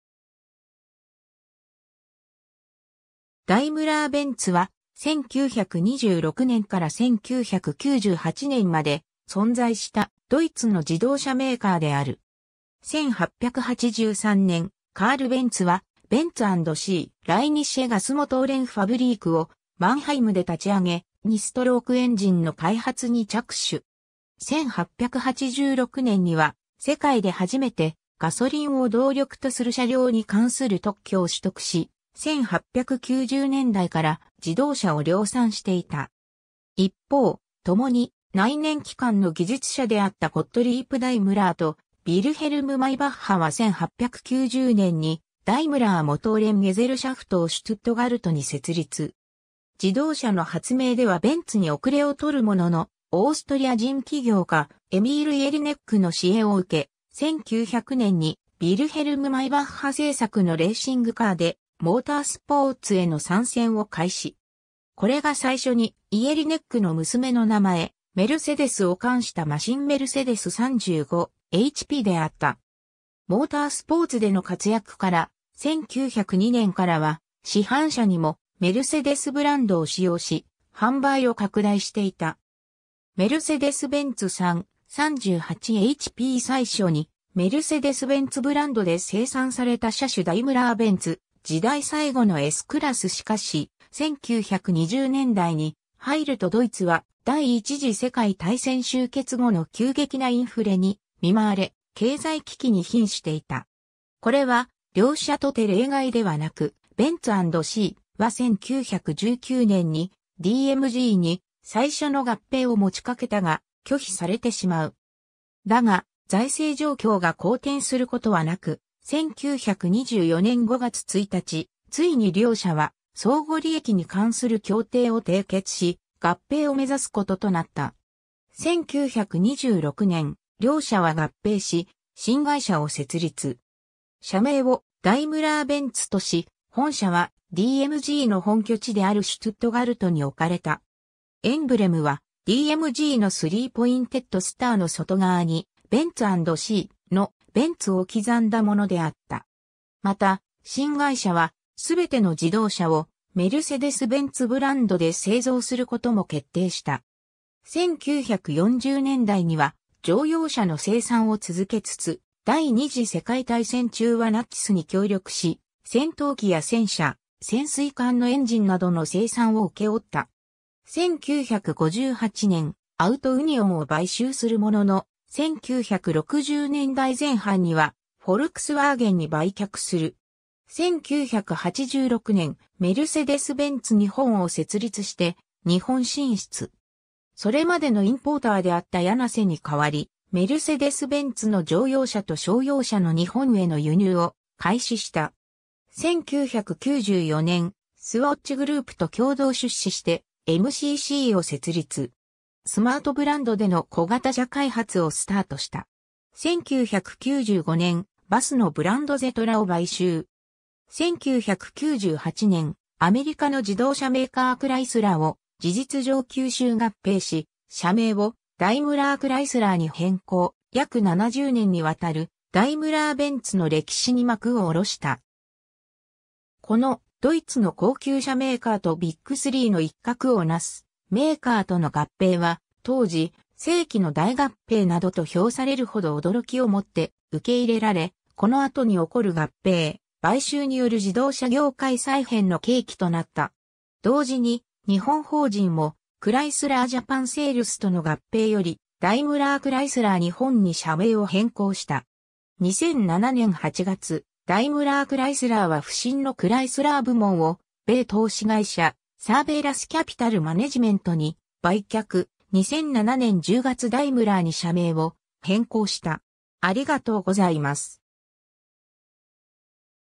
ダイムラー・ベンツ
ドイツの自動車メーカー・ブランド
カール・ベンツ
ダイムラー・ベンツ（Daimler-Benz AG）は、1926年から1998年まで存在したドイツの自動車メーカーである。
 1883年、カール・ベンツはベンツ& Cie. ライニッシェ・ガスモトーレン・ファブリーク（以降、ベンツ& Cie.）をマンハイムで立ち上げ、2ストロークエンジンの開発に着手。
1886年には世界で初めて「ガソリンを動力とする車両」に関する特許を取得し、1890年代から自動車を量産していた。
 一方、ともに内燃機関の技術者であったゴットリープ・ダイムラーとヴィルヘルム・マイバッハは1890年にダイムラー・モトーレン・ゲゼルシャフト（以降、DMG）をシュトゥットガルトに設立。
自動車の発明ではベンツに後れを取るものの、オーストリア人企業家エミール・イェリネックの支援を受け、1900年にヴィルヘルム・マイバッハ制作のレーシングカーでモータースポーツへの参戦を開始。
これが最初にイェリネックの娘の名前「メルセデス」を冠したマシンメルセデス・35hpであった。
モータースポーツでの活躍から、1902年からは市販車にもメルセデスブランドを使用し、販売を拡大していた。
 メルセデス・ベンツ 3/38hp（1927年） 最初にメルセデス・ベンツブランドで生産された車種 ダイムラー・ベンツ時代最後のSクラス(W140) しかし、1920年代に入るとドイツは第一次世界大戦終結後の急激なインフレに見舞われ、経済危機に瀕していた。
これは両社とて例外ではなく、ベンツ& Cie.は1919年にDMGに最初の合併を持ちかけたが、拒否されてしまう。
だが、財政状況が好転することはなく、1924年5月1日、ついに両社は「相互利益に関する協定(Agreement of Mutual Interest)」を締結し、合併を目指すこととなった。
 1926年、両社は合併し、新会社を設立。
社名をダイムラー・ベンツとし、本社はDMGの本拠地であるシュトゥットガルトに置かれた。
エンブレムはDMGのスリー・ポインテッド・スターの外側にベンツ& Cie.の「BENZ」を刻んだものであった。
また、新会社はすべての自動車をメルセデス・ベンツ（DMGの「メルセデス」とベンツ& Cie.の「ベンツ」をとった）ブランドで製造することも決定した。
 1940年代には乗用車の生産を続けつつ、第二次世界大戦中はナチスに協力し、戦闘機や戦車、潜水艦のエンジンなどの生産を請け負った。
 1958年、アウトウニオン（現アウディ）を買収するものの、1960年代前半にはフォルクスワーゲンに売却する。
 1986年、メルセデス・ベンツ日本（MBJ）を設立して日本進出。
それまでのインポーターであったヤナセに代わり、メルセデス・ベンツの乗用車と商用車の日本への輸入を開始した。
 1994年、スウォッチ・グループと共同出資してMCC（Micro Car Corporation ）を設立。
スマートブランドでの小型車開発をスタートした。
 1995年、バスのブランドゼトラを買収。
 1998年、アメリカの自動車メーカークライスラーを事実上吸収合併し、社名をダイムラー・クライスラーに変更、約70年にわたるダイムラー・ベンツの歴史に幕を下ろした。
このドイツの高級車メーカーとビッグスリーの一角をなすメーカーとの合併は当時「世紀の大合併」などと評されるほど驚きを持って受け入れられ、この後に起こる合併・買収による自動車業界再編の契機となった。
同時に日本法人もクライスラー・ジャパンセールスとの合併よりダイムラー・クライスラー日本に社名を変更した。
 2007年8月、ダイムラー・クライスラーは不振のクライスラー部門を米投資会社、サーベラス・キャピタル・マネジメントに売却、2007年10月ダイムラーに社名を変更した。